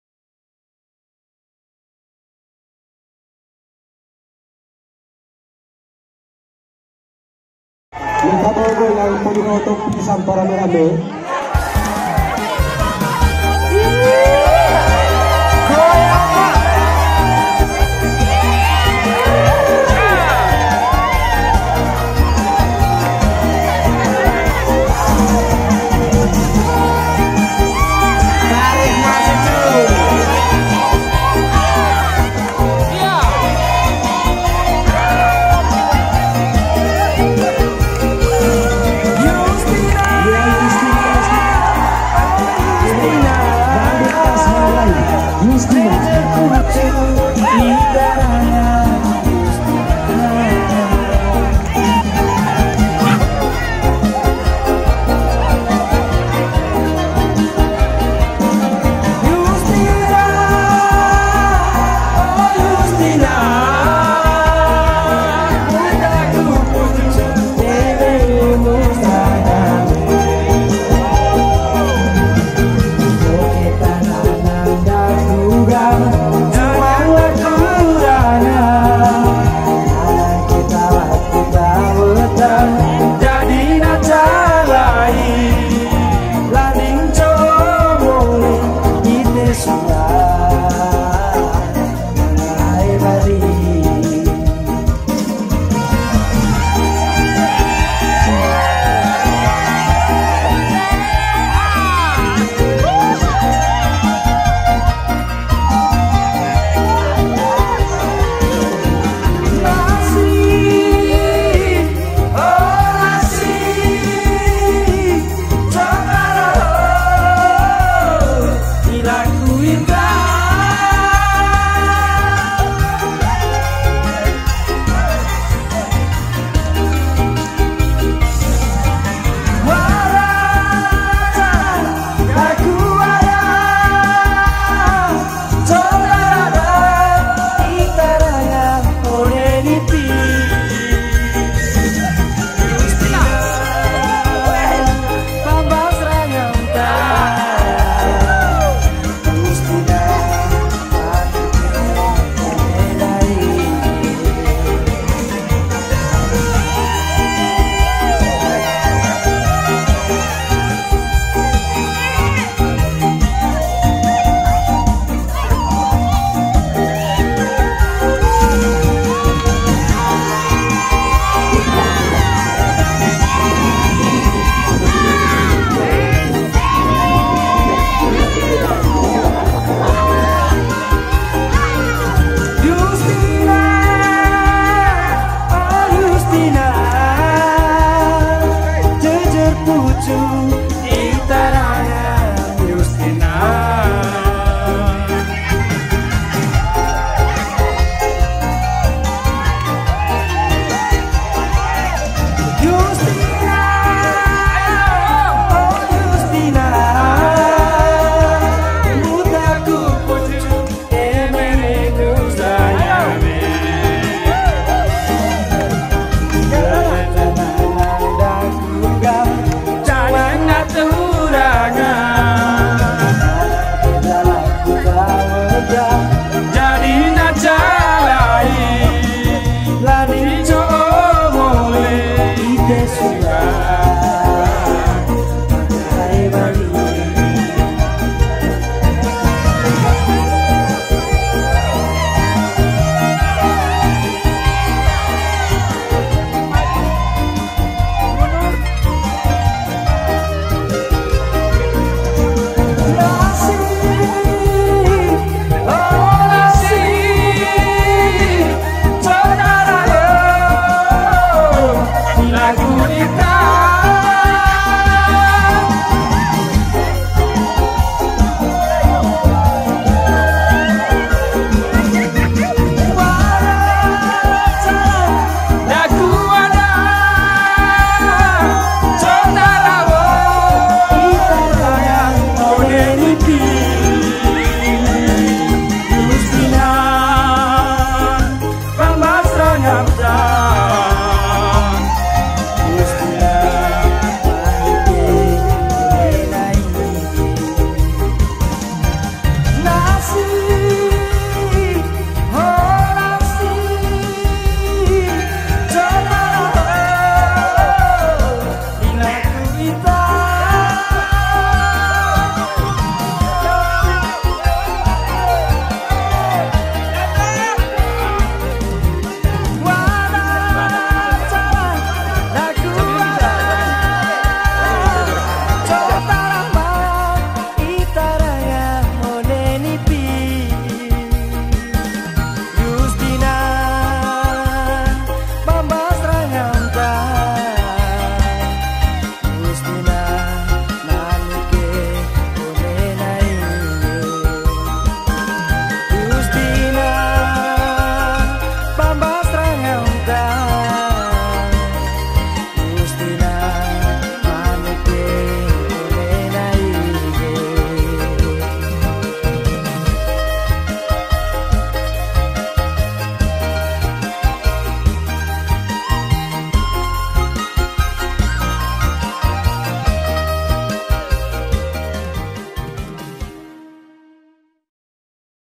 Festival Budaya Manggarai Jakarta 2019 "Yustina"
Festival budaya Manggarai Jakarta 2019. Acara ini digagaskan oleh Komunitas Perempuan Manggarai (KPM). Acara ini memperkenalkan budaya Manggarai seperti Tarian Caci, Tarian Rangkuk Alu, Makanan khas Manggarai, dan masih banyak lagi. Acara berlangsung dua hari yaitu Sabtu 17 Agustus dan 18 Agustus 2019. Acara ini dihadiri Gubernur DKI Jakarta, Anies Rasyid Baswedan, Ph.D. Turut hadir pula tokoh tokoh Manggarai di Jakarta, DR. Benny K. Harman,SH,MH; Sebastian Salang, Gabriel Mahall, Johny G. Plate. Turut hadir Pula Uskup Bogor Mgr. Paskalis Bruno Syukur, OFM.
Acara ini berlangsung di Anjungan NTT Taman Mini Indonesia Indah (TMII).